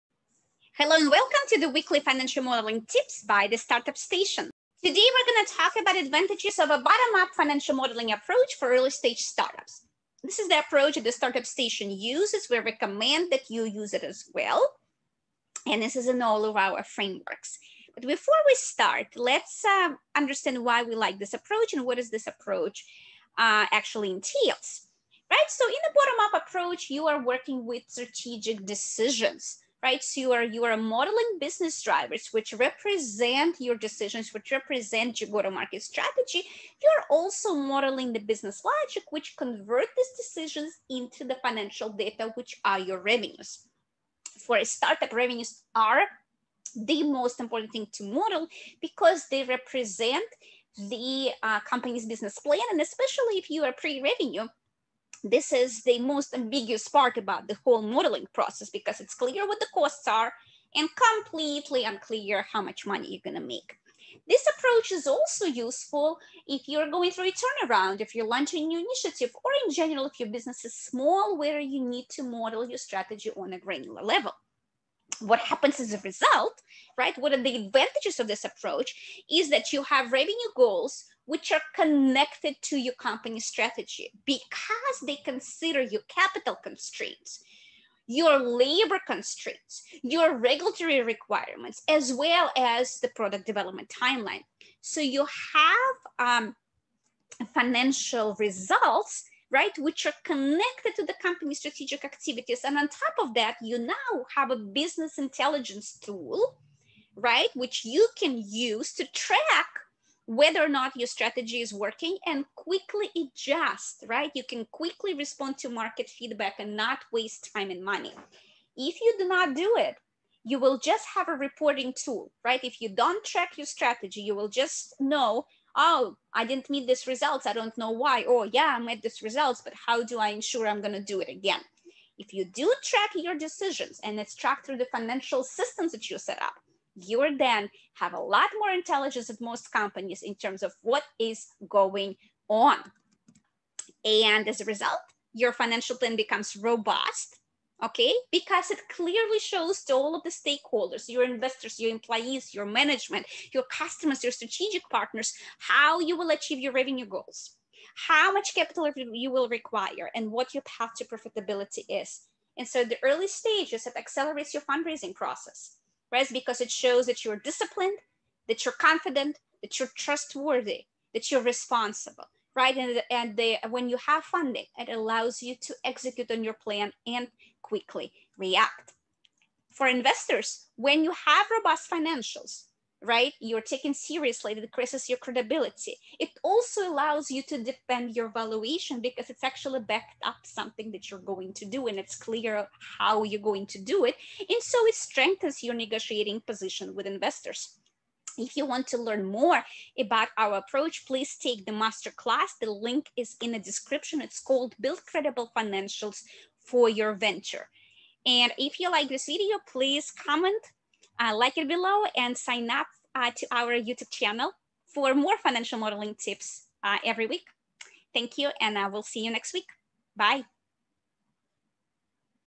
Advantages of bottom-up modeling for Early-Stage Startups: Financial Modeling Tip
In this video, The Startup Station's founder and President, Victoria Yampolsky, discusses why the bottom-up financial modeling approach, when applied to startups correctly, produces a credible financial plan and allows you to create financials investors will take seriously.

We cover how to formulate assumptions and how to model each scenario in Excel. This is applicable to any company, including a pre-revenue startup.